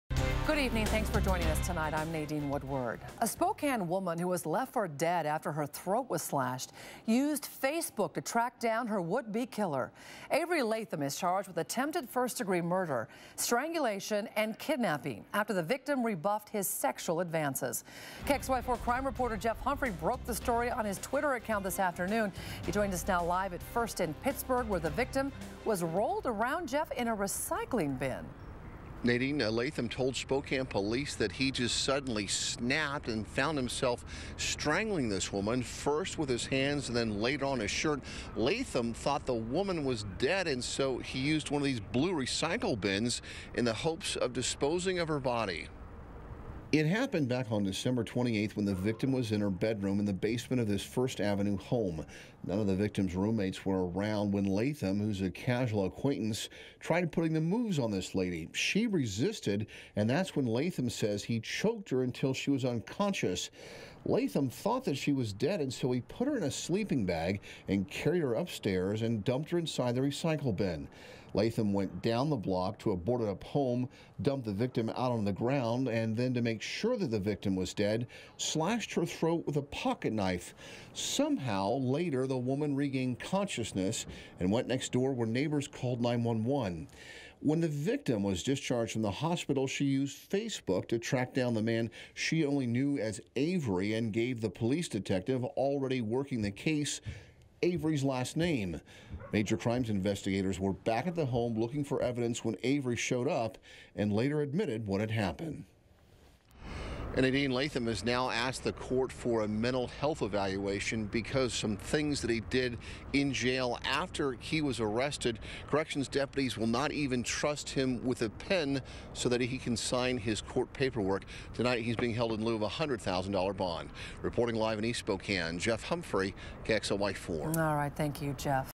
Spokane woman tracks down would-be killer via Facebook
A Spokane woman who was left for dead after her throat was slashed used Facebook to track down her would-be killer. KXLY 4's Jeff Humphrey reports.